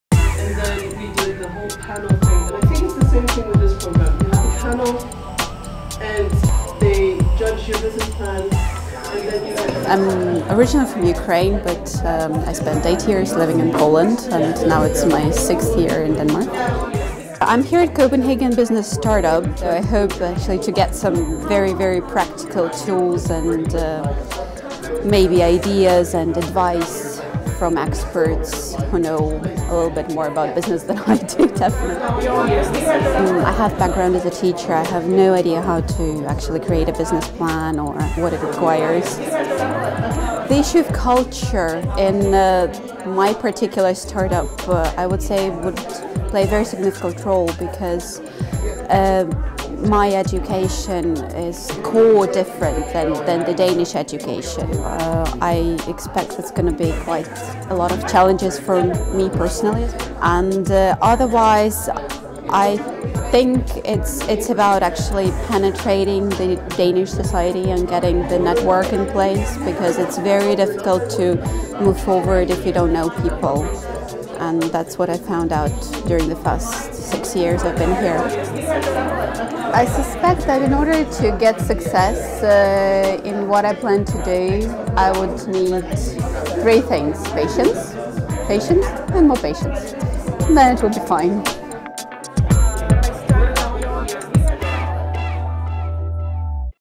Copenhagen Business Startup, Introduction Lecture
Copenhagen Business Startup program.

Making Business Plan, SWOT analysis, Financial plan. Etc.